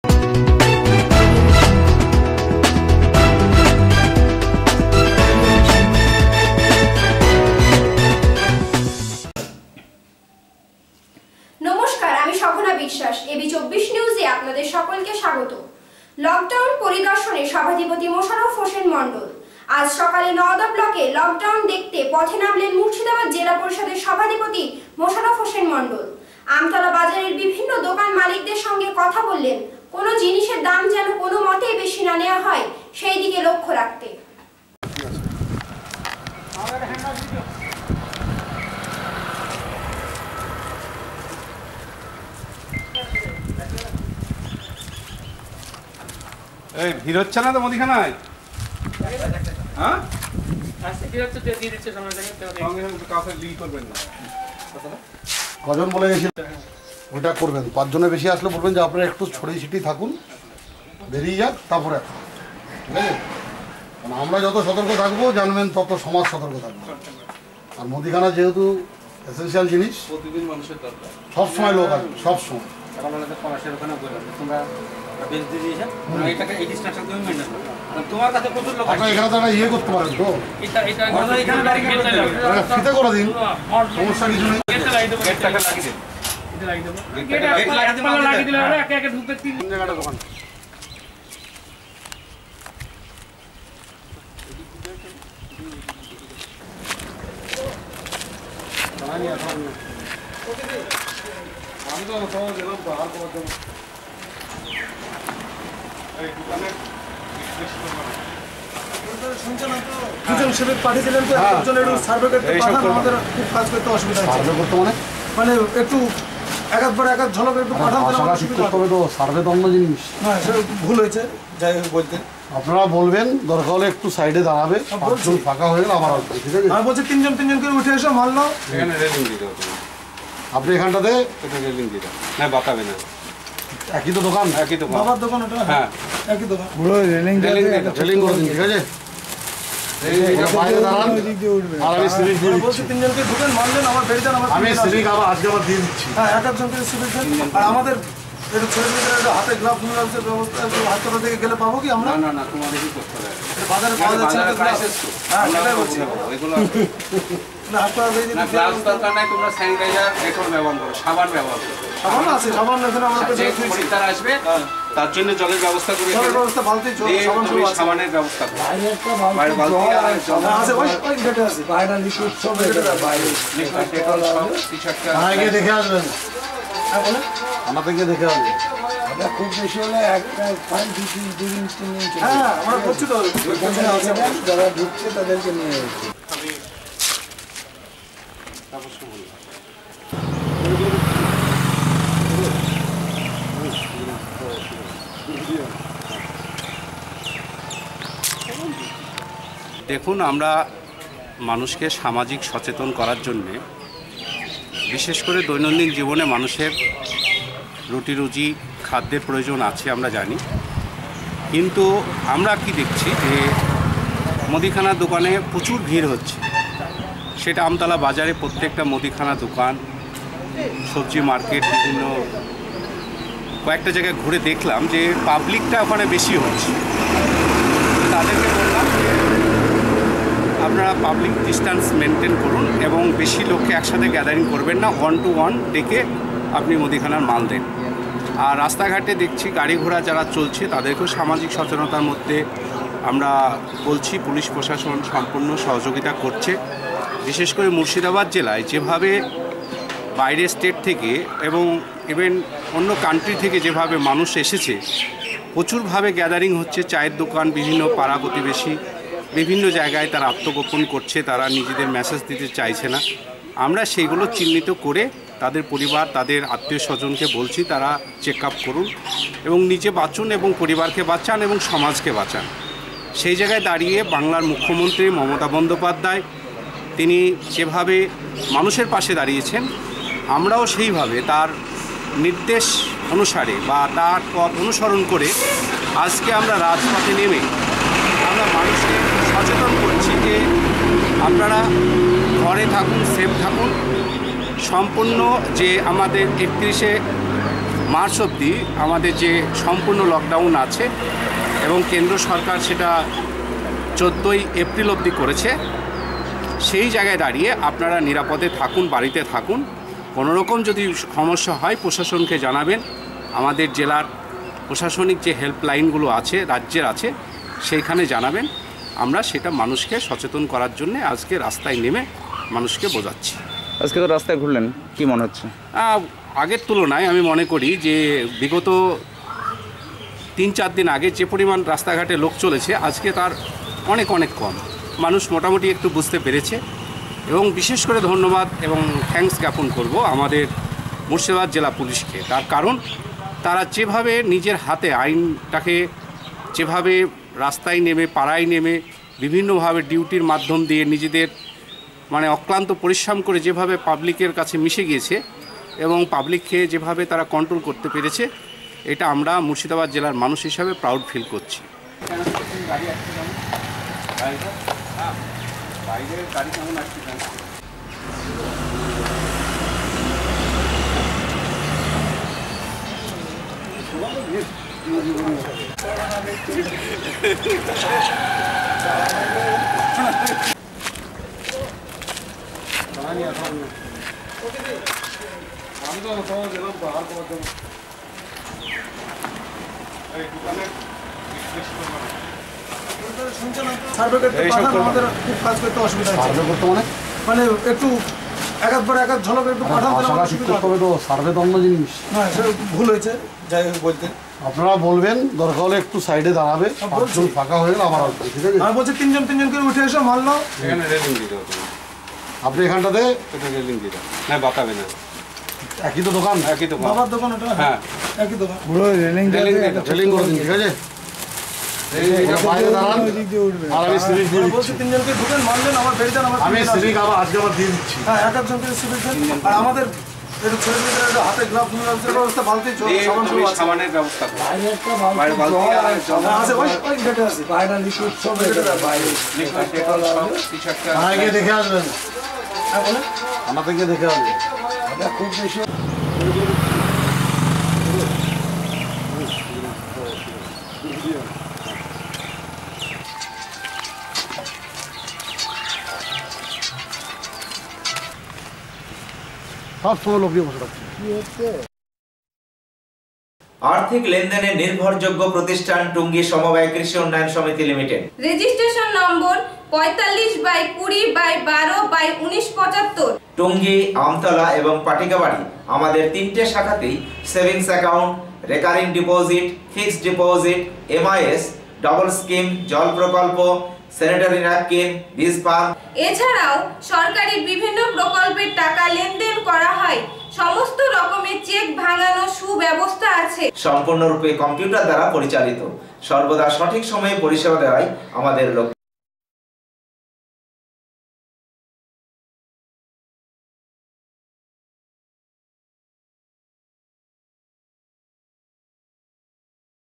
।।লকডাউন পরিদর্শনে সভাধিপতি মোশারফ হোসেন মন্ডল।।নওদা।।AB24NEWS#
।।লকডাউন পরিদর্শনে সভাধিপতি মোশারফ হোসেন মন্ডল।।নওদা।।
PRESIDENT MOSAROFF HOSSAIN MONDAL VISITED THE LOCKDOWN